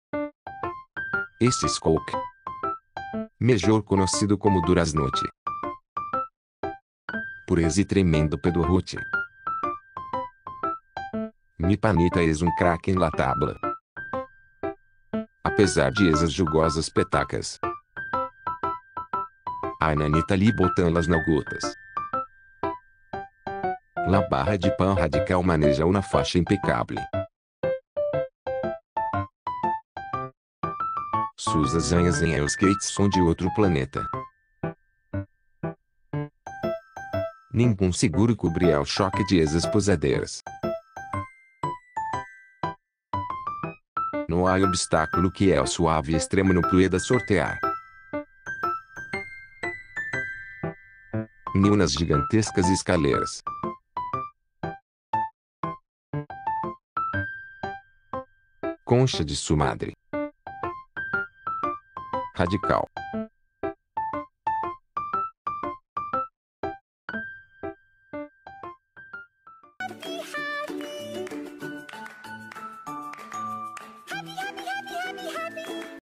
Corgi Nalgón es un Crack con la Tabla Bajando Escaleras
Coke es un tremendo crack en el skate, la barra de pan radical puede bajar cientos de escaleras controlando la tabla como un profesional.

[DY@  屁桃可乐]